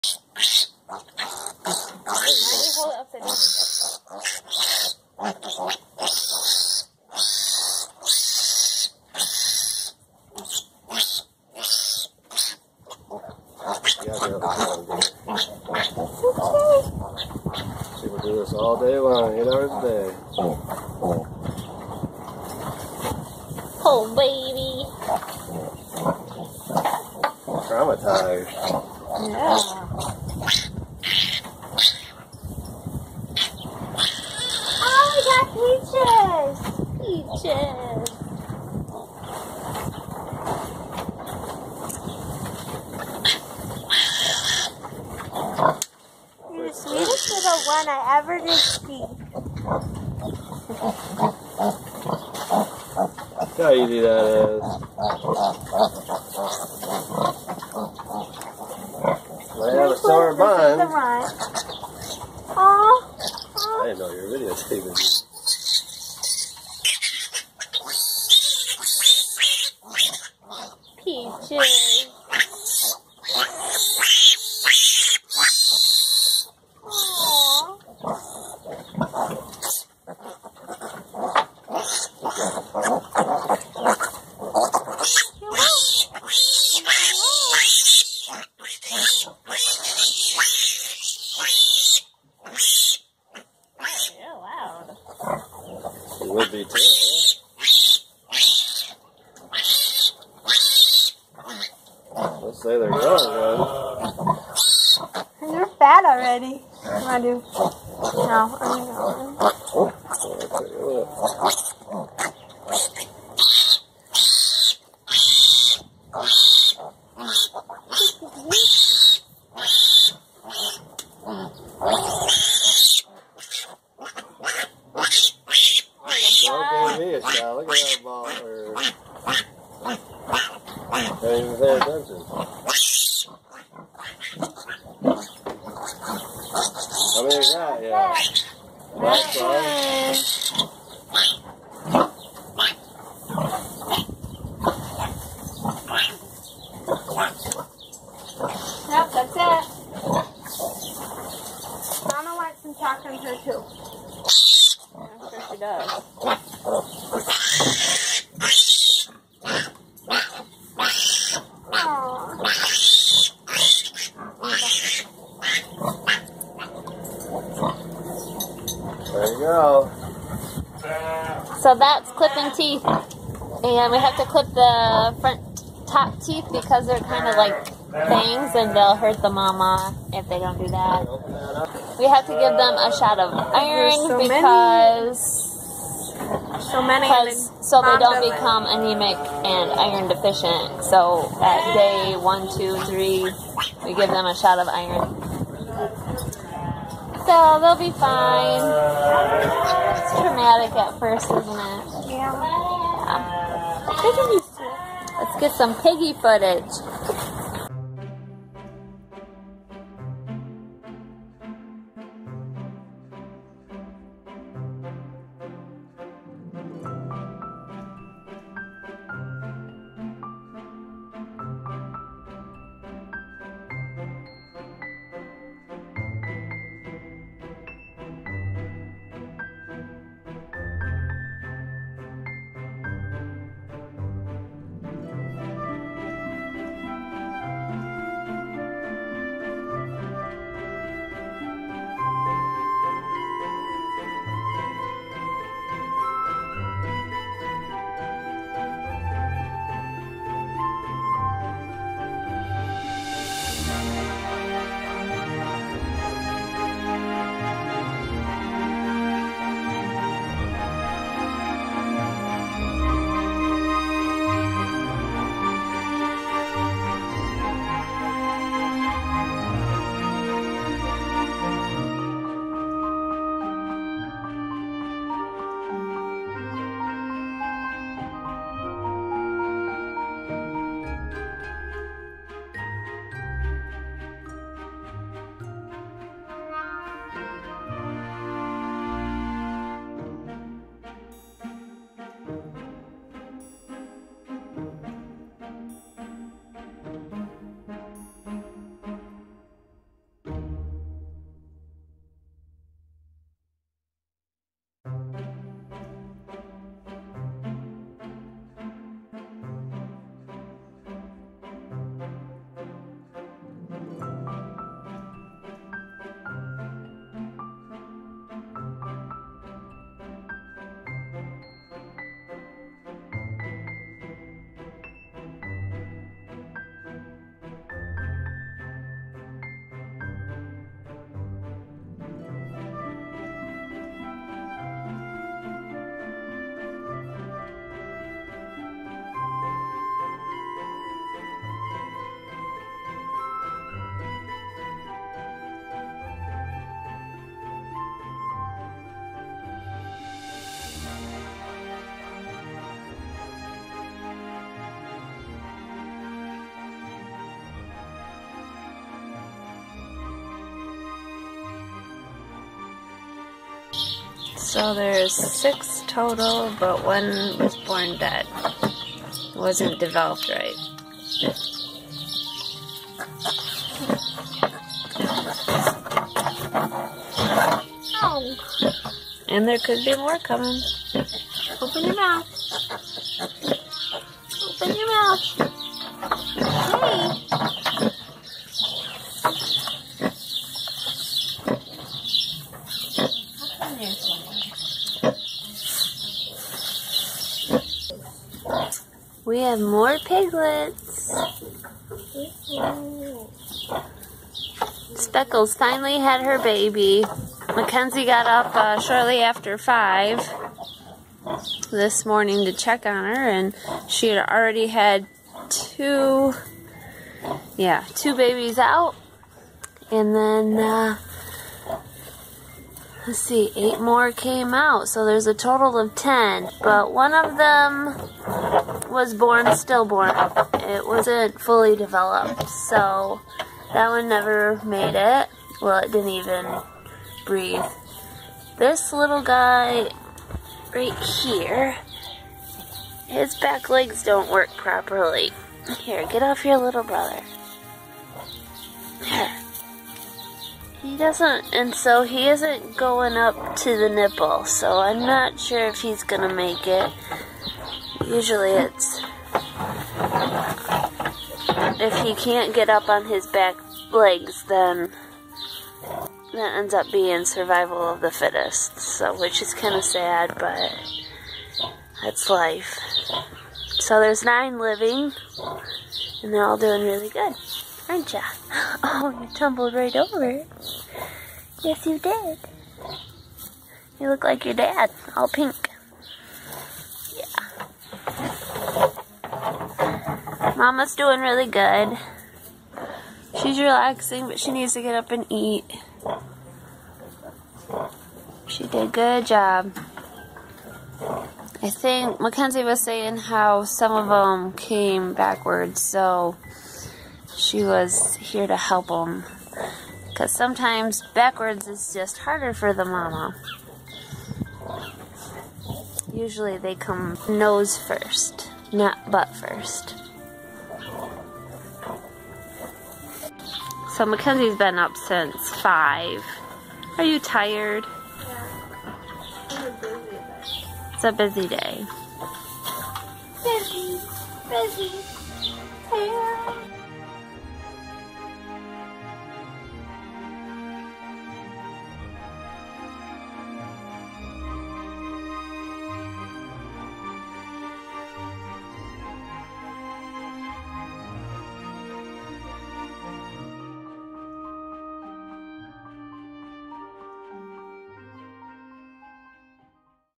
Speckle Finally Gives Birth and Trimming Needle Teeth
acres of clay homestead